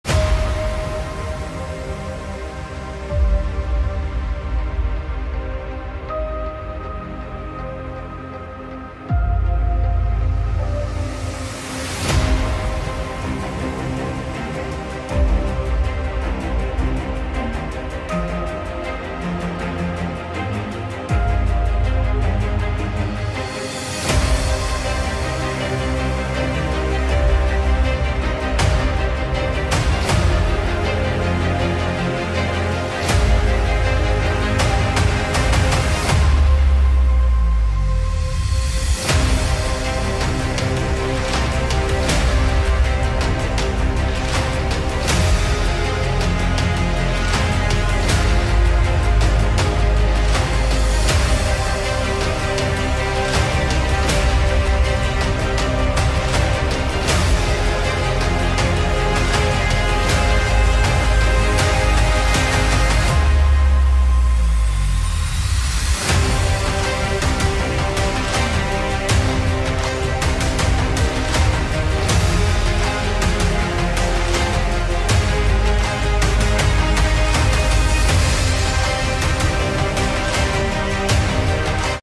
Messi and Argentina at the FIFA WORLD CUP QATAR Winning World Cup Trophy! WhatsApp status
Messi and Argentina won the FIFA WORLD CUP QATAR 2022 and Messi was holding the World Cup trophy. WhatsApp Status. Football legend Messi is the GOAT and Scored 3 Goals against Mbappe in France. As seen in the Qatar World Cup announcement. Messi finished his last World Cup with a huge triumph.